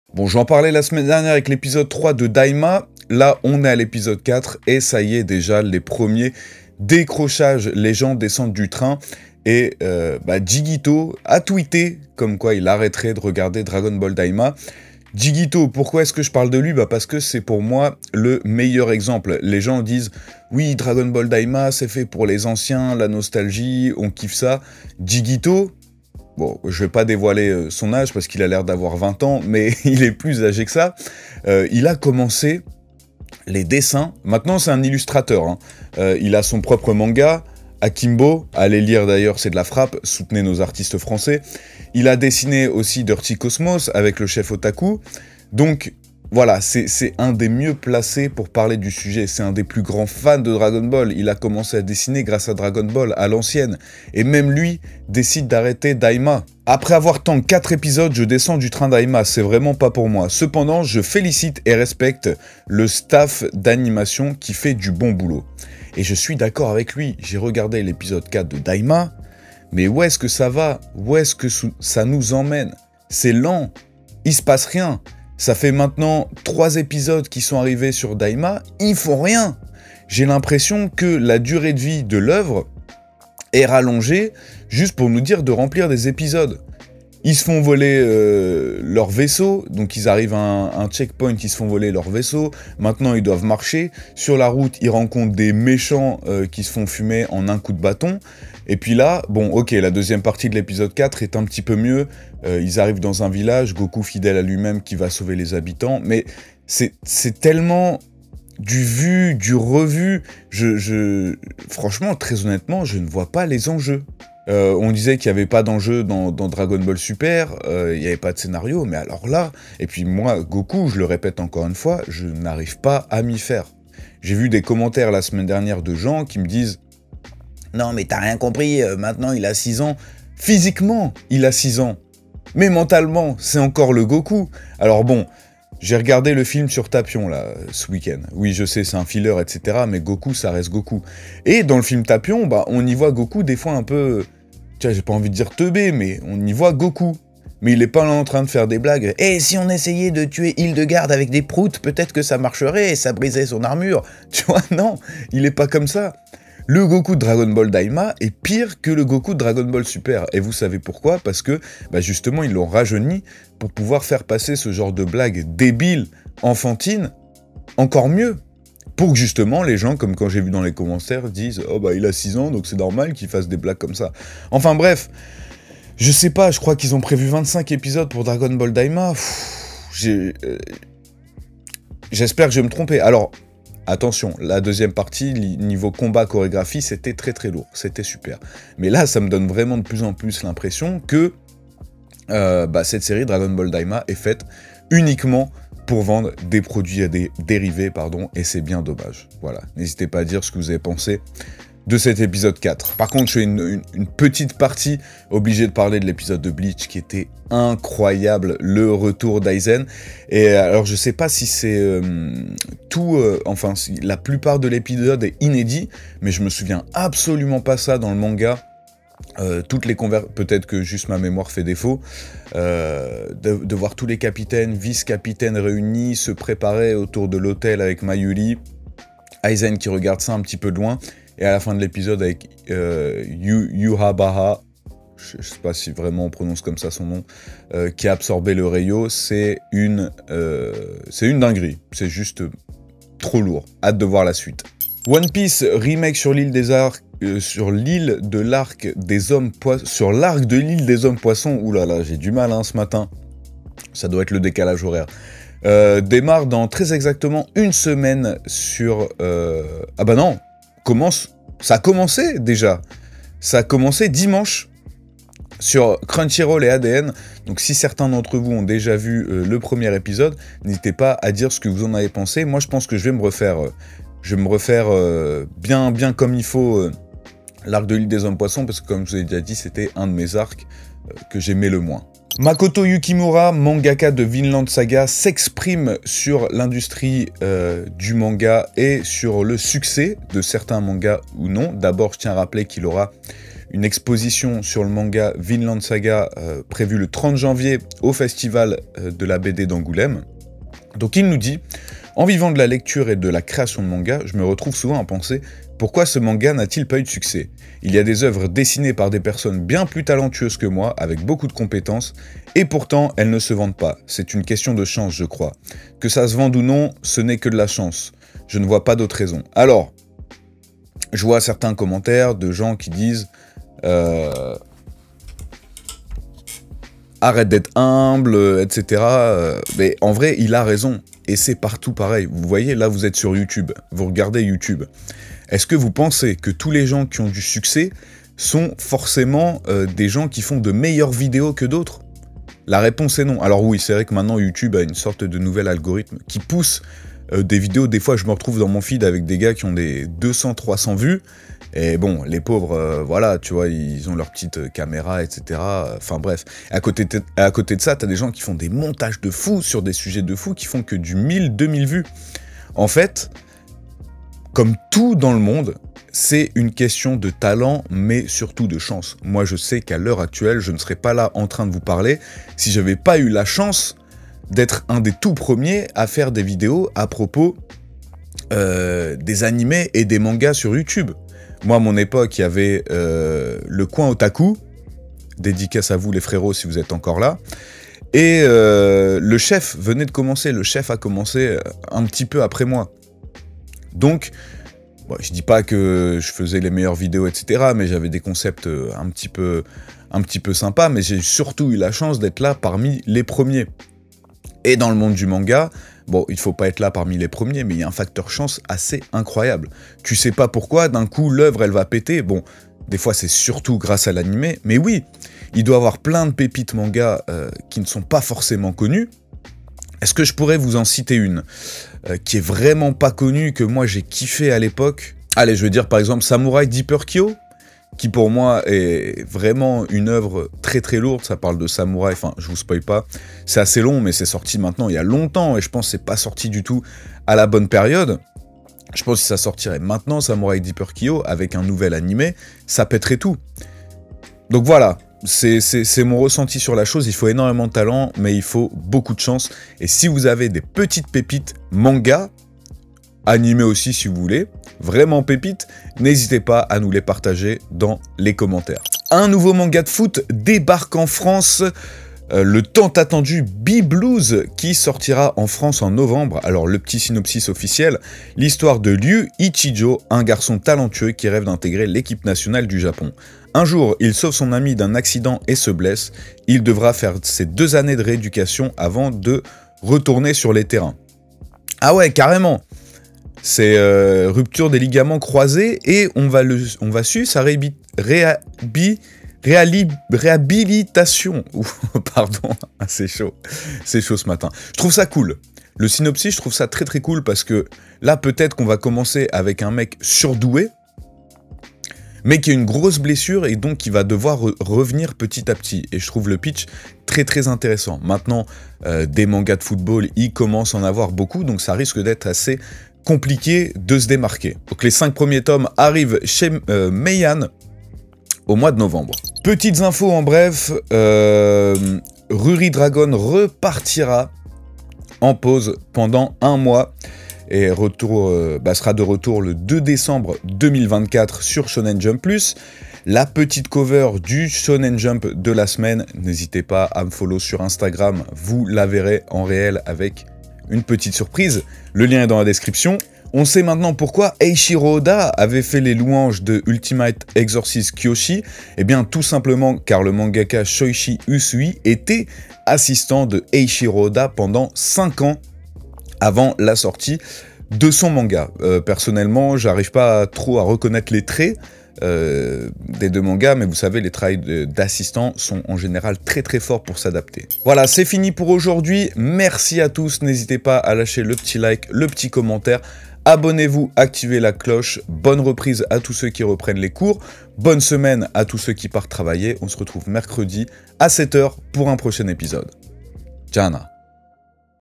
Un Mangaka dit les TERMES sur Dragon Ball Daima ! POURQUOI certain Manga ne Percent pas ?
Les news otaku du jour en moins de 10 minutes c'est Le Réveil Otaktu tous les lundi , mercredi et vendredi a 7h du Matin !

00:00 - Dragon Ball Daima ca commence quand ?
03:23 -  Le retour d'Aizen dans BLEACH QUEL REGAL
04:05 - L'anime ONE PIECE de retour
04:43 : Le succes des Manga : Talent Ou Chance ?
07:39 : L'actu En Bref